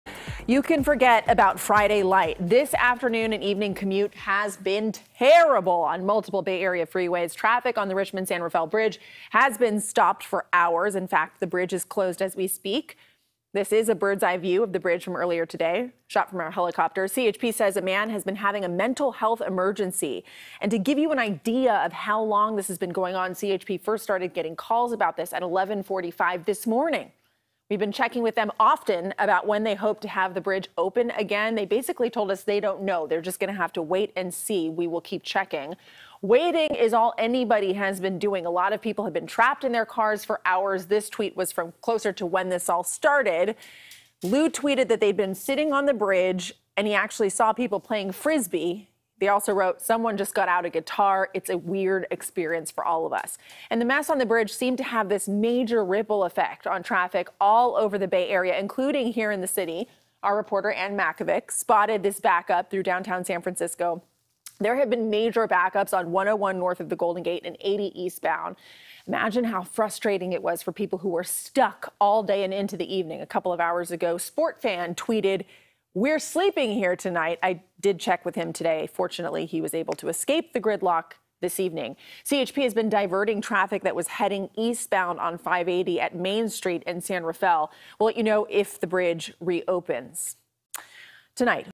Eastbound Richmond-San Rafael Bridge closed for man suffering mental crisis
The CHP says a man having a mental health emergency is the reason eastbound traffic was stopped all afternoon and evening on the Richmond San Rafael Bridge Friday. (7-21-23)